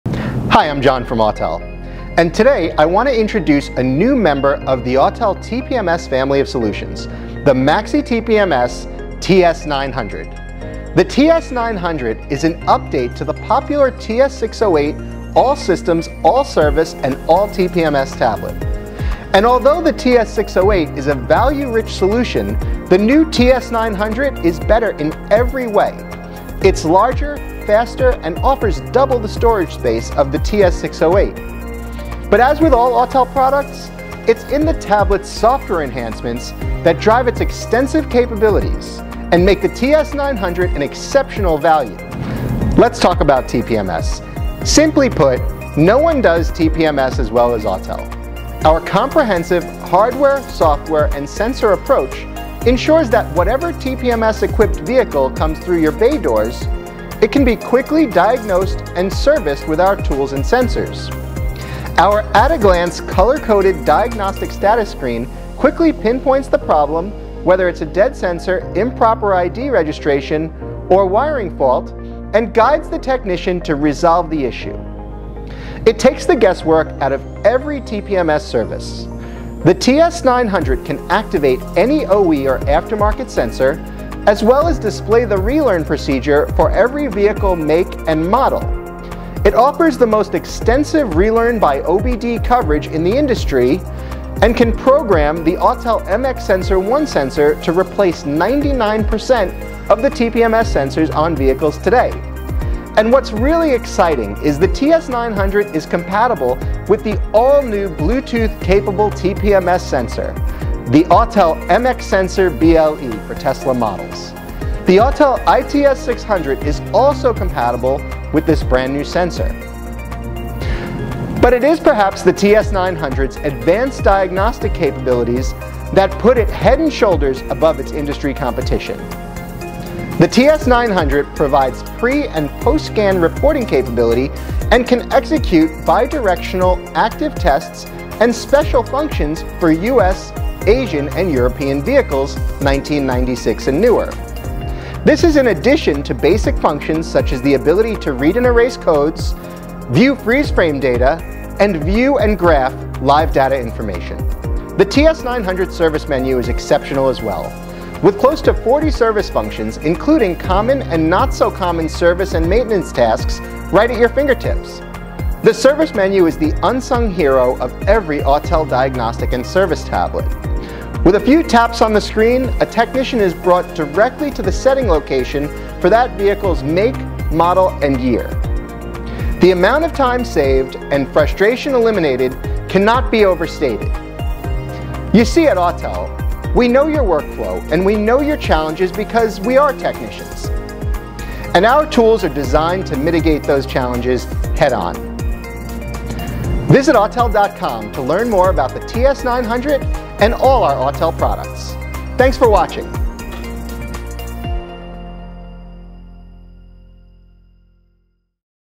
Autel Enhances your TPMS Service with the MaxiTPMS TS900
The MaxiTPMS TS900 is an 8" TPMS, Diagnostics and Service, touchscreen tablet. The TS900 can activate, read and relearn all known TPMS sensors (including Tesla OE sensors) and program the Autel 1-Sensor to replace 99% of the TPMS sensors on the road today.

The TS900 is also compatible with the Autel Bluetooth-TPMS sensors (BLE-A001), preprogrammed for Tesla models and programmable for BLE-equipped vehicles. Diagnostic functions include all systems Pre/Post Scans, bi-directional Active Tests, and Special Functions. The TS900 offers an advanced processor, 4GB operating memory, 64GB storage memory and runs on Android 11.